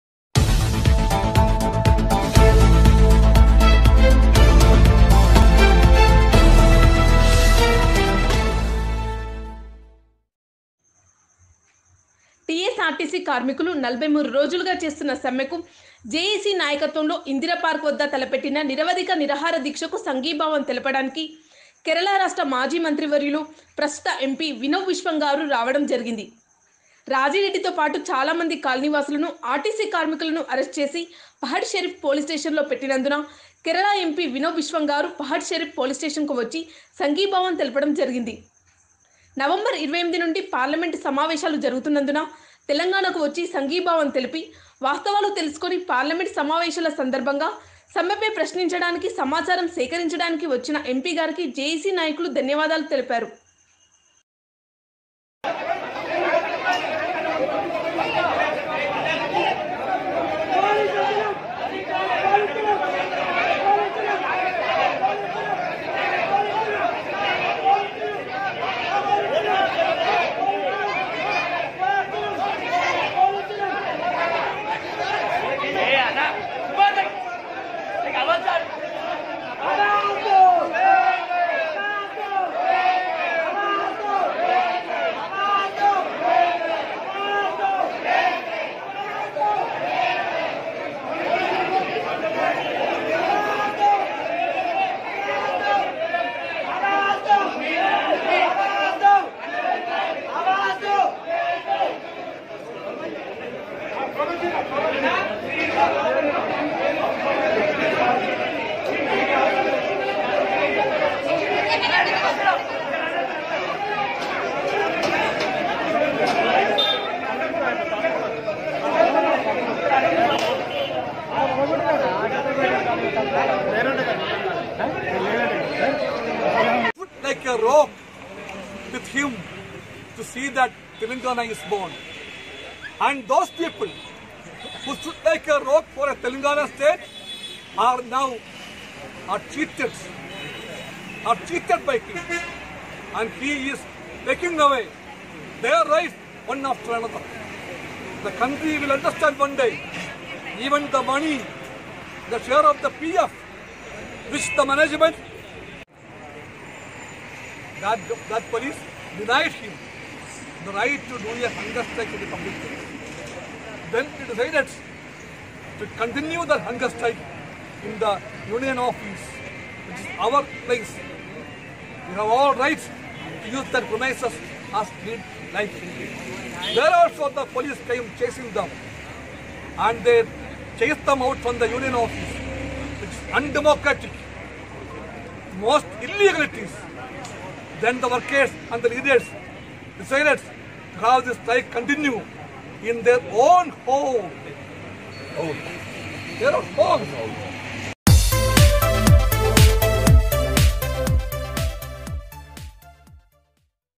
ఆర్టీసీ కార్మికులు చేస్తున్న సమ్మెకు మద్దతు తెలిపిన కేరళ ఎంపీ వినవ్ బిశ్వం గారు | V News Telangana
టిఎస్ఆర్టీసి కార్మికులు 43 రోజులు గా చేస్తున్న సమ్మె కు మరియు జెఎసి నాయకత్వం ఇ0దిరా పార్క్ వద్ద జెఎసి నాయకత్వం తలపెట్టిన నిరవధిక నిరాహారదీక్షకూ సంఘీభావం తెలుపటానికి కేరళ రాష్ట్ర మాజీ మంత్రి వర్వులు ప్రస్తుత MP వినవ్  బిశ్వంగారు రావడం జరిగింది.
    రాజి రెడ్డి తో పాటు చాల మంది కాలనీ వాసులను ఆర్టీసీ కార్మికులను  మ అరెస్టు చేసి పహాడి షరీఫ్ పోలీస్ స్టేషన్ లో పెట్టినందున  కేరళ.MP CPI  వినవ్ బిశ్వం గారు పహాడి షరీఫ్ పోలీస్ స్టేషన్ కు వచ్చి సంఘీభావం తెలపడం జరిగింది.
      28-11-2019  నుండి పార్లమెంట్ సమావేశాలు జరుగుతున్నందున తెలంగాణ కు వచ్టి సంఘీభావం తెలిపి వాస్తవాలు తెలుసుకుని పార్లమెంట్ సమావేశాల సందర్భంగా సమ్మె పై ప్రశ్నించడాని సమాచారం సేకరించడానికి వచ్చిన MP గారికి జెఎసి దన్వవాధములు తెలుపుతుంది.
కె.రాజి రెడ్డి జెఎసి కో కన్వీనర్